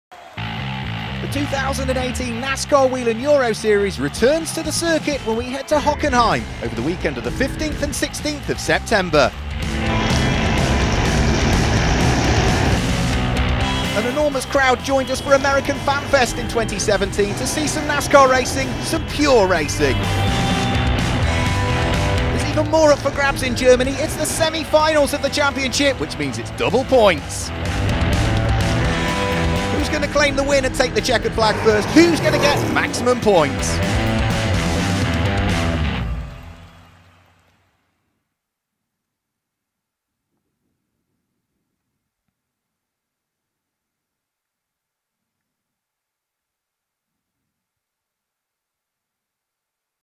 NASCAR GP GERMANY 2018 | Teaser
Official teaser for the 2018 American Fan Fest at the Hockenheimring, Germany.